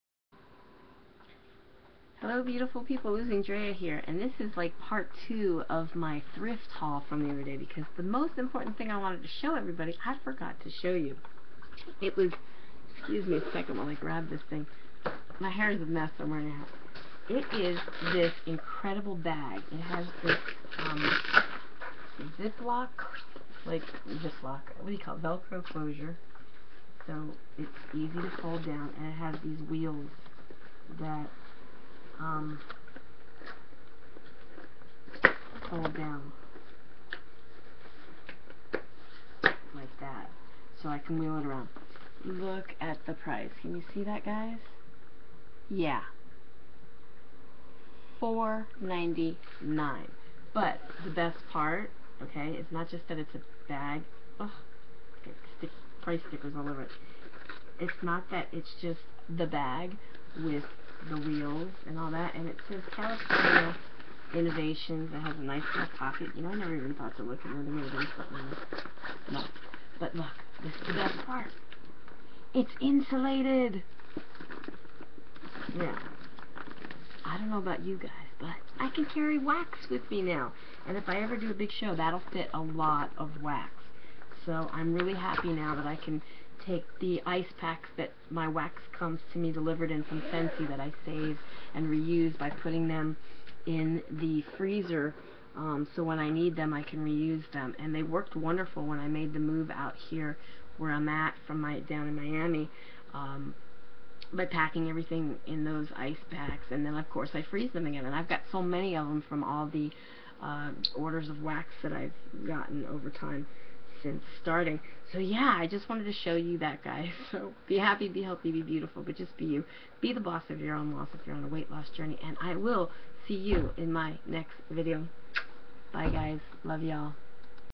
Thrift Store Haul II
This is from the same haul I did on Sunday however I forgot to show you the best thing I got aside from the Ott-Lite of course which by the way JoAnn's Fabrics and Crafts has that same lamp now on 50% off sale for $89.99 Down from $179.99 so WOW did I get an awesome deal for only $19.99!!! That for sure was a Mega Jackpot Thrift Find!

~Hugs~
Drea